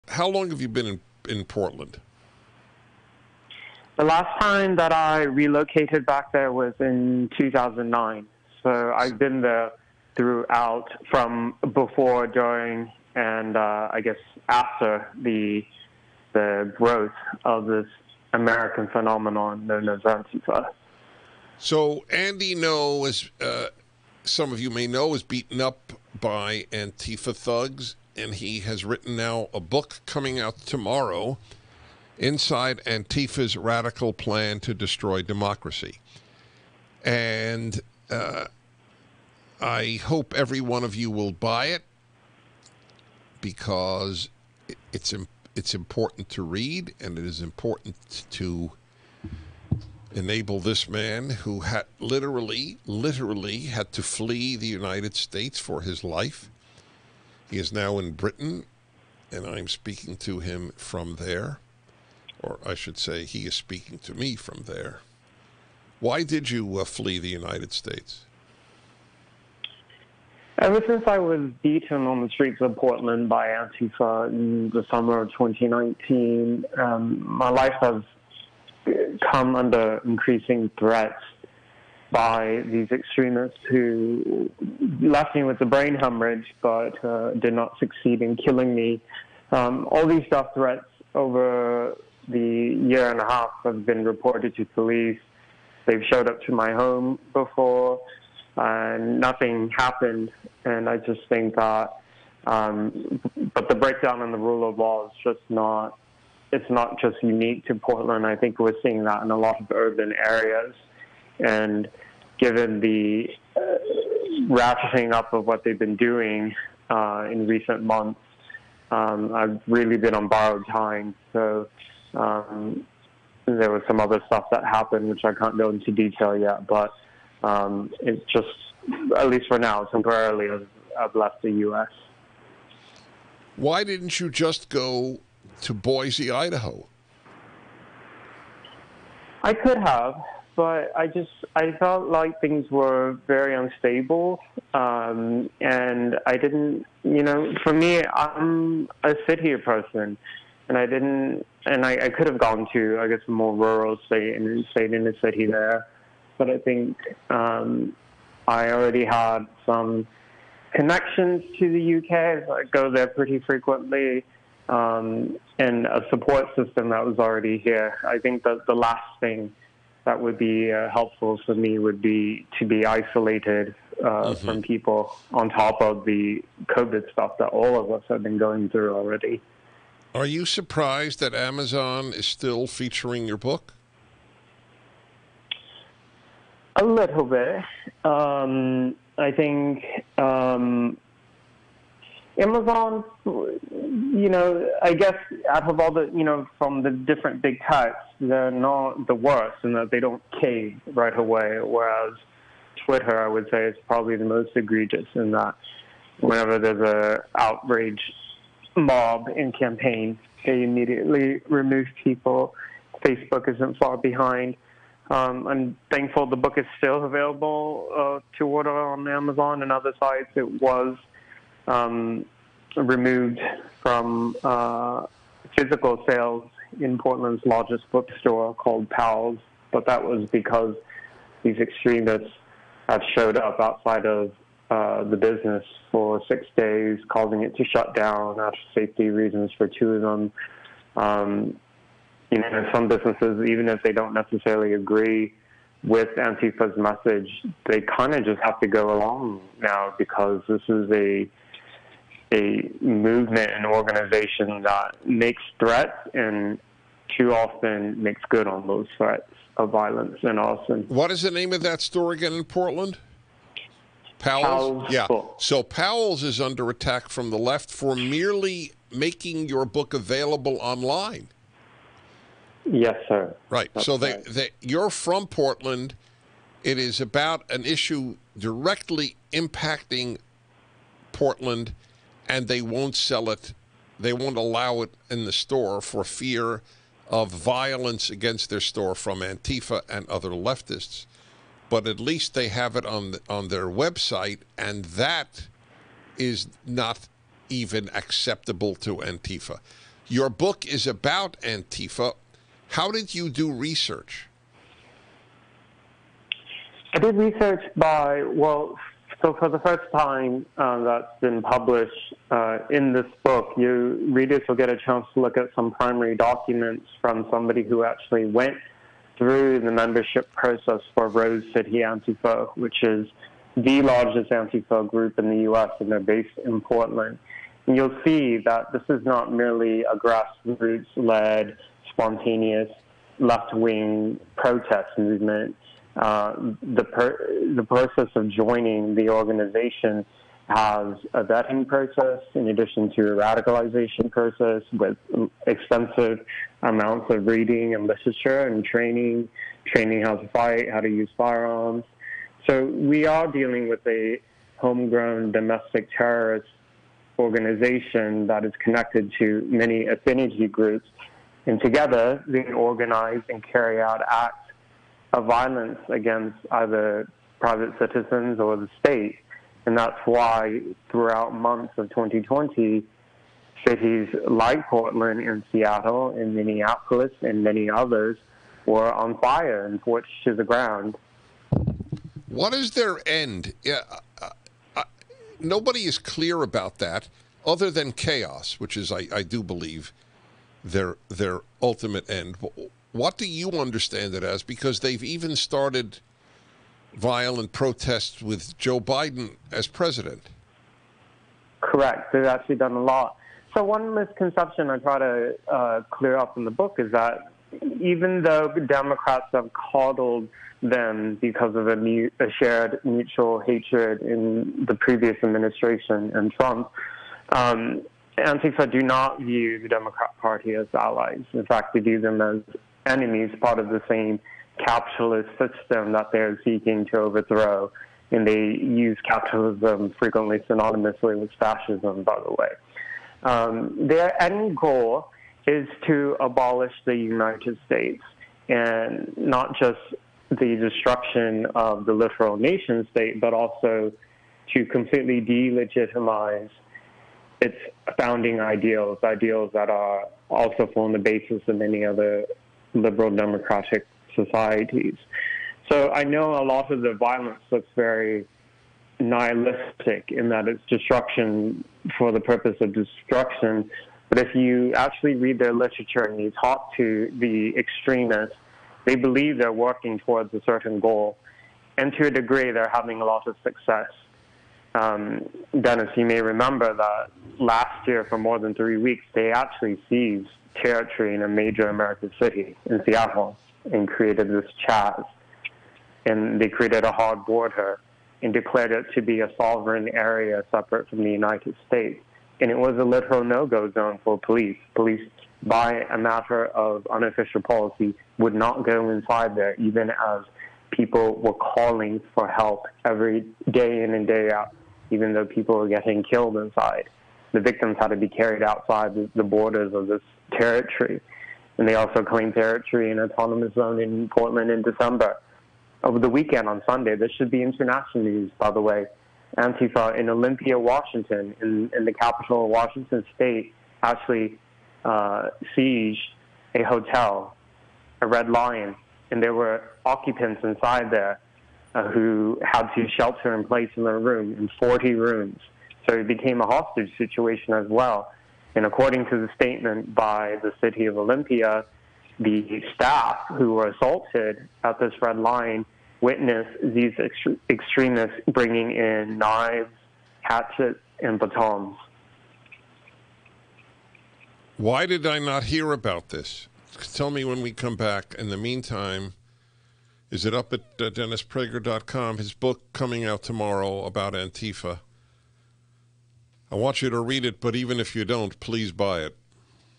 Inside Antifa's Radical Plan to Destroy Democracy
Andy Ngo talks to Dennis about his brand new book, "Unmasked."